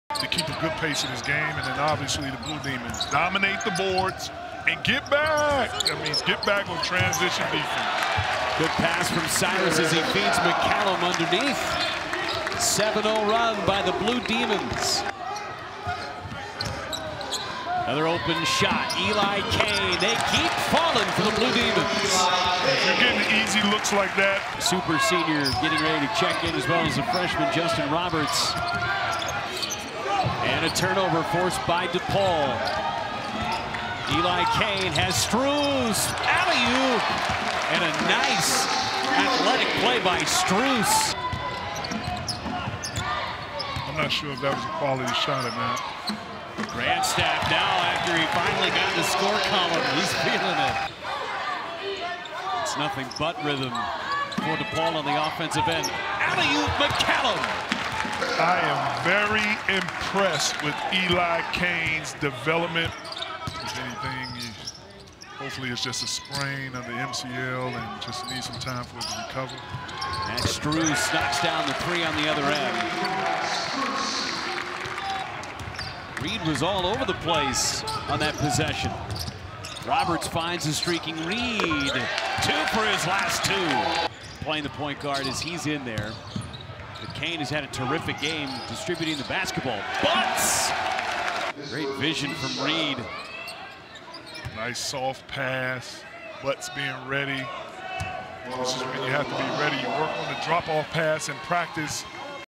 DePaul vs Alabama A&M | Highlights | FOX COLLEGE HOOPS
Max Strus leads DePaul with 14 points as they beat Alabama A&M 83-60.